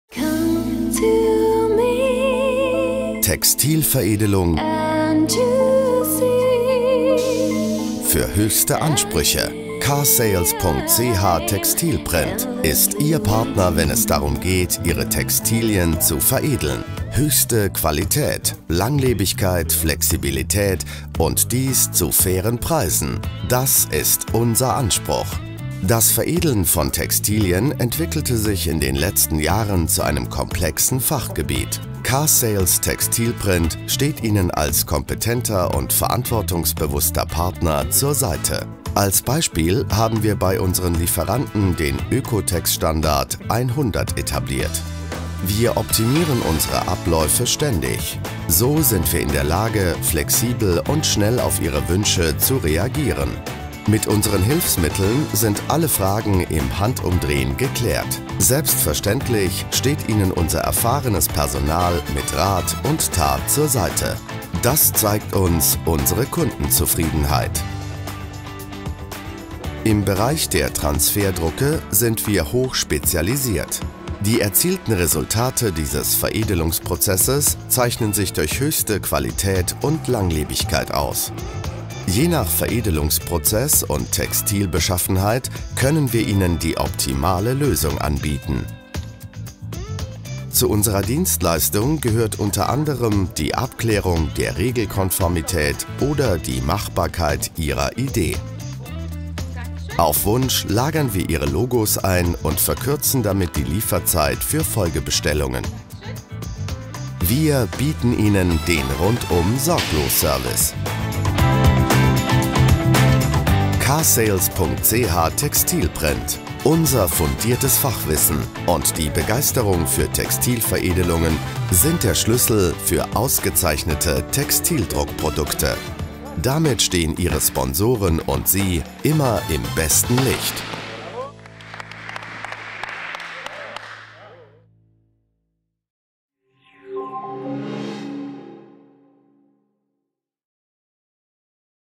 k-sales.ch Transferdruck -- Textilveredelung für höchste Ansprüche!
Höchste Qualität, langlebig, flexibel, kostengünstig
Das Veredeln von Textilien entwickelte sich in den letzten Jahren zu einem komplexen Fachgebiet.

Neben dem Direktdruckverfahren hat sich insbesondere im professionellen Bereich der Transferdruck durchgesetzt.

Bei diesem indirekten Druckverfahren werden Logos, Schriften, Bilder oder Texte nicht direkt auf die zu veredelnden Textilien gedruckt, sondern zuerst auf eine Transferfolie gedruckt und dann erst mit einer Thermotransferpresse übertragen.

Die erzielten Resultate zeichnen sich durch höchste Qualität und Langlebigkeit aus und eignen sich sowohl für Einzelstücke wie auch für grössere Stückzahlen. Weitere Vorteile sind die Lagerfähigkeit der Transfers, die Möglichkeit der Drucke auf Abruf und die Schnelligkeit in der Fertigung.

Transferdruck -- Textilveredelung für höchste Ansprüche!